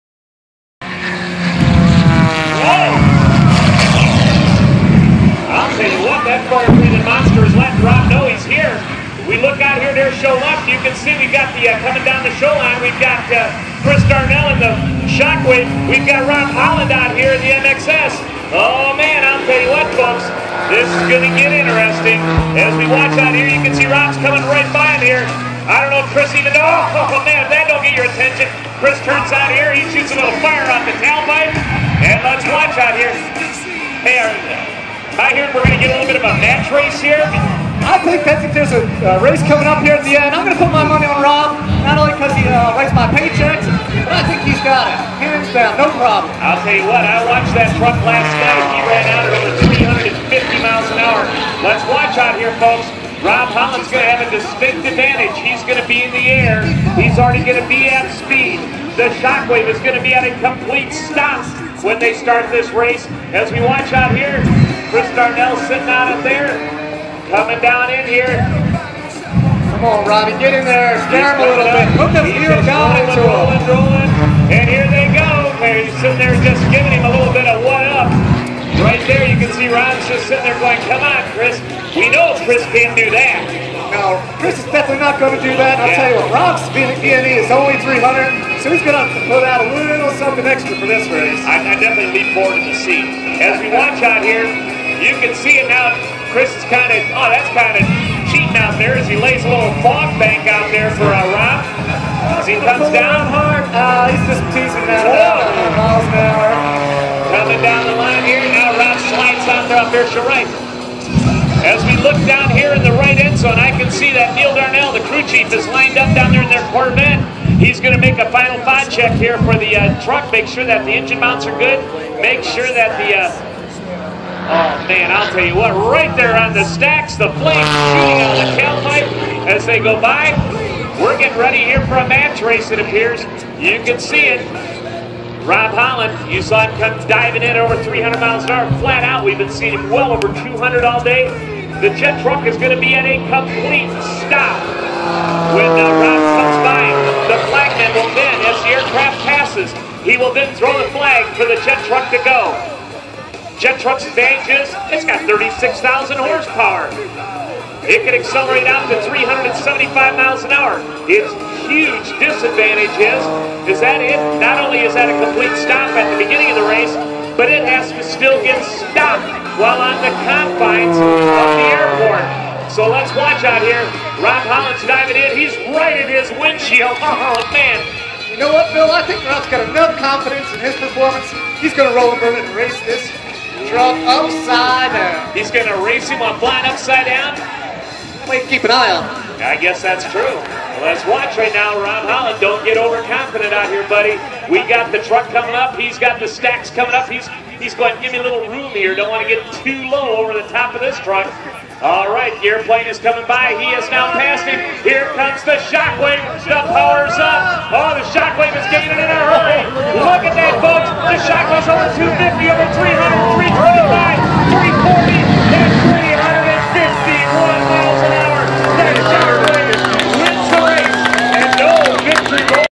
Schock Wave vs Plane at 2013 Battle Creek Air Show
Semi vs Plane